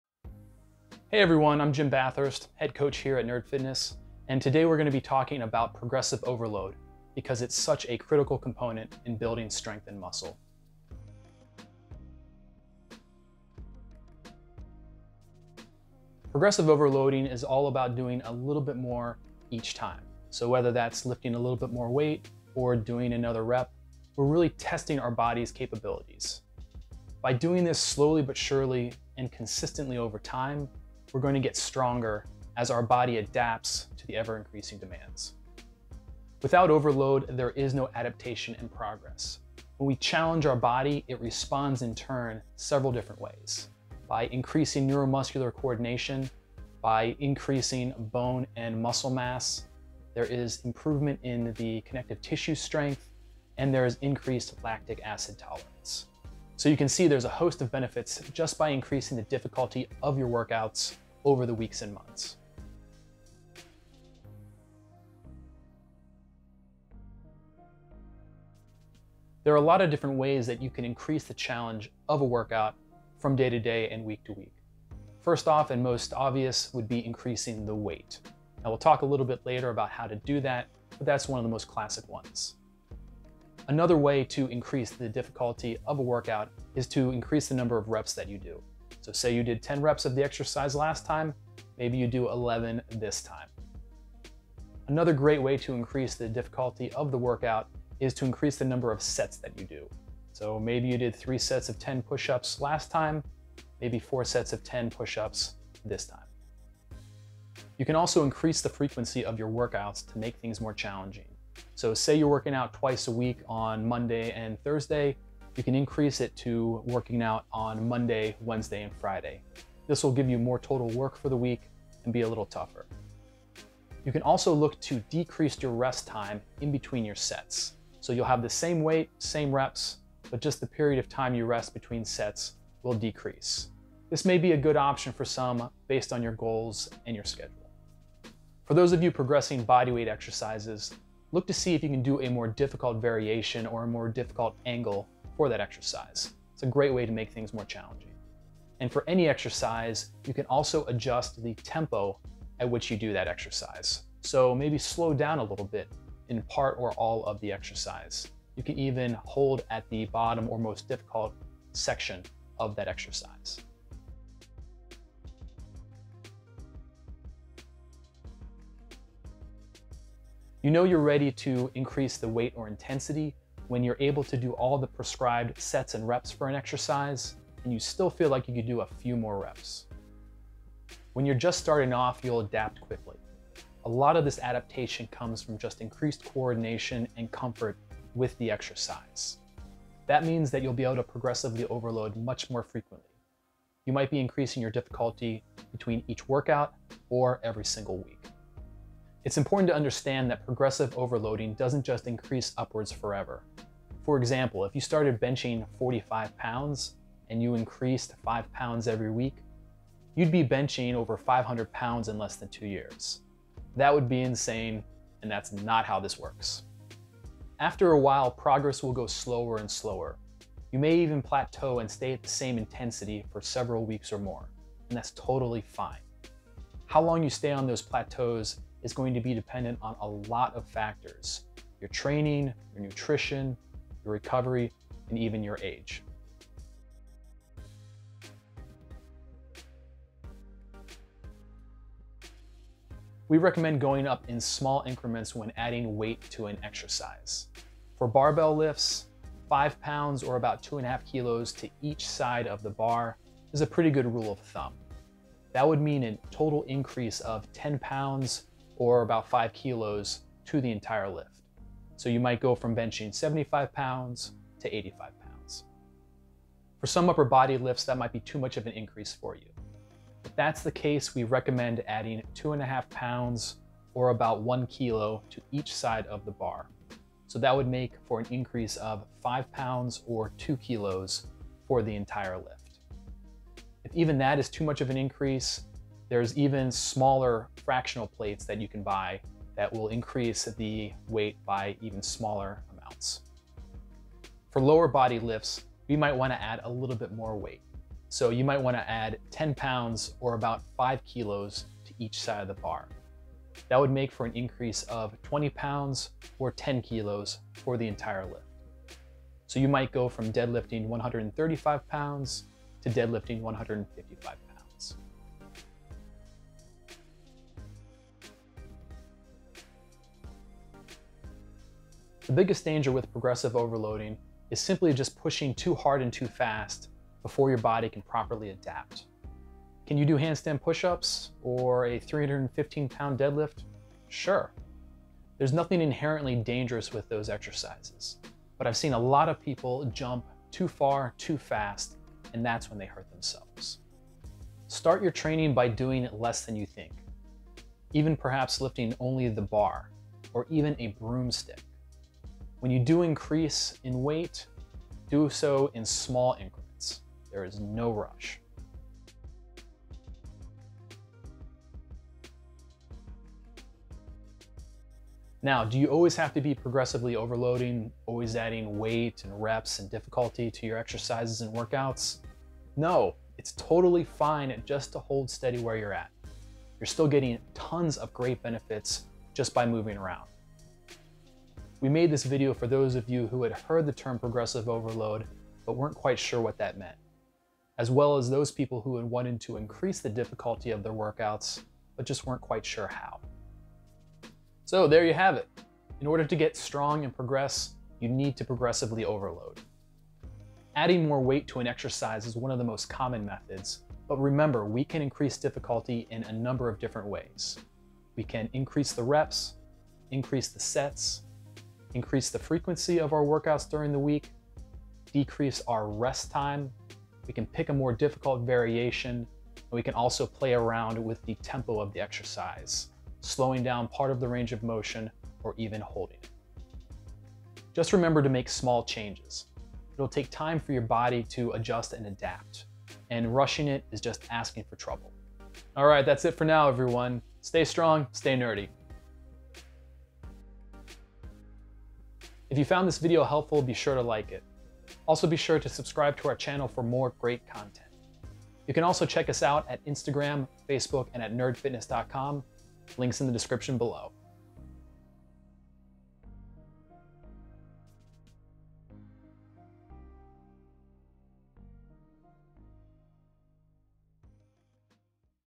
What is Progressive Overload? (+ How to Do It)
This video will teach you everything you need to know about progressive overload.

Have you ever asked yourself the question, “What is progressive overload?”

If so, then this video is for you!

Here are some of the things we’ll cover today:

0:00, Intro!

0:16, we’ll explain progressive overload and why it’s important for strength training.

1:09, we’ll discuss eight different ways to follow a progressive overload strategy. Yes, you could just start lifting more and more weight. However, there are seven other great strategies to use too.

We’ll discuss all of them.

2:50, we’ll cover how to tell when you’re ready for progressive overload.

4:02, will find us discussing how much weight you should be adding week over week.

5:22, brings us to possible concerns of progressive overload.

6:05, will answer the question on whether you even need to follow a progressive overload strategy.